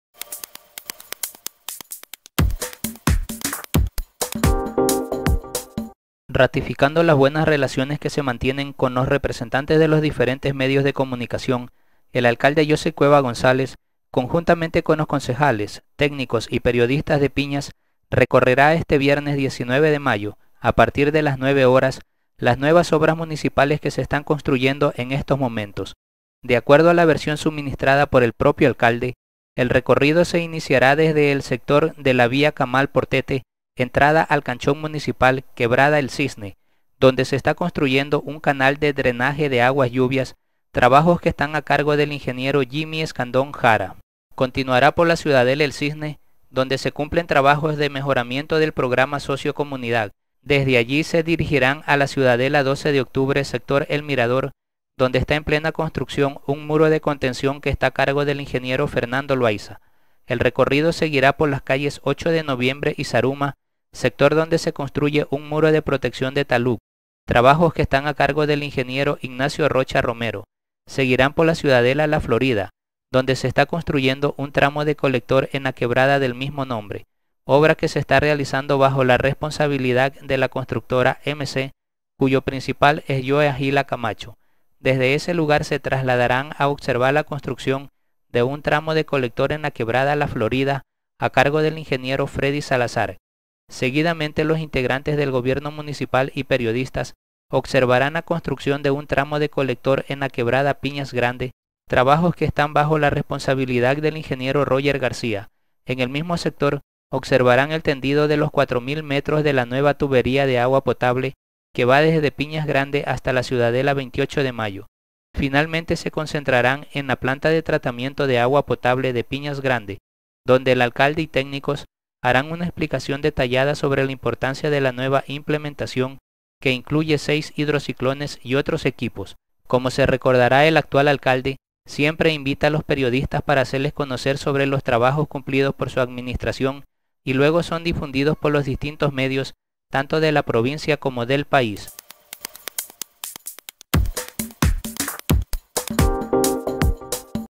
Alcalde , concejales y periodistas recorreran nuevas obras
Ratificando las buenas relaciones que se mantienen con los representantes de los diferentes medios de comunicación, el Alcalde Joseph Cueva González conjuntamente con los concejales, técnicos y periodistas de Piñas recorrerá este viernes (19) a partir de las 09H00 las nuevas obras municipales que se están construyendo en estos momentos